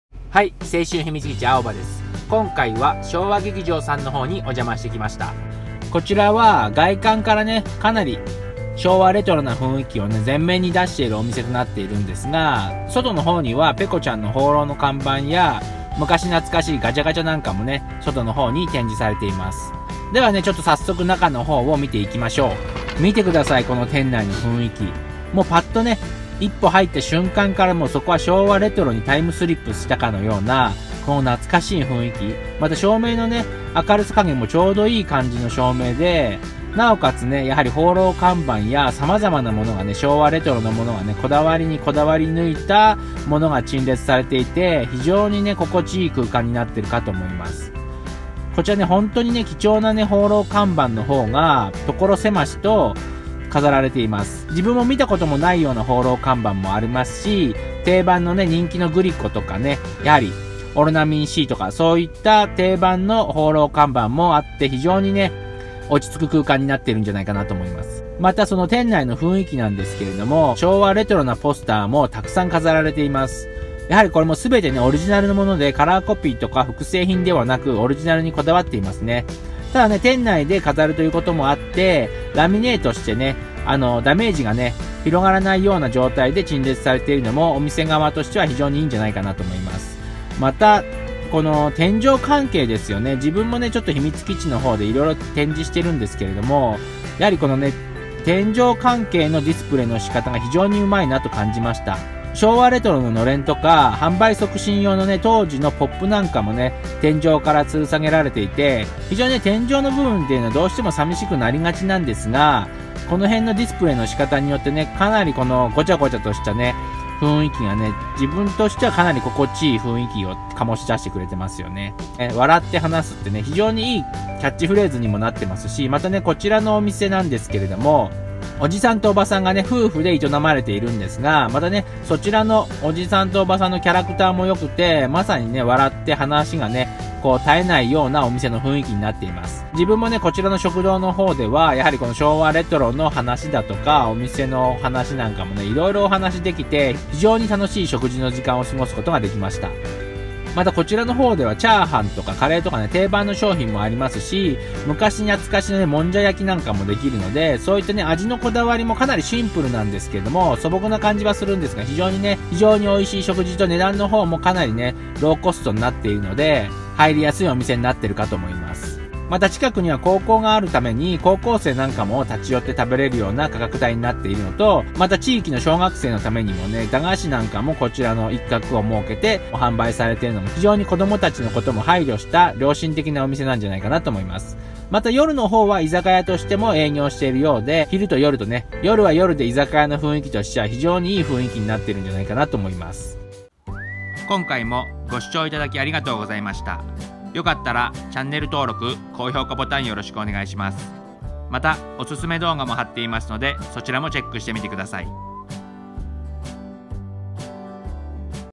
【笑話劇場】昭和レトロな食堂、笑話劇場さんにおじゃましました。昭和一色のこだわりの店内は雰囲気抜群です。
昭和レトロな食堂、笑話劇場さんにおじゃましました。昭和一色のこだわりの店内は雰囲気抜群です。
青春秘密基地チャンネル紹介動画はコチラ↓
青春秘密基地は大人の趣味を掘り下げて動画をアップしていきます。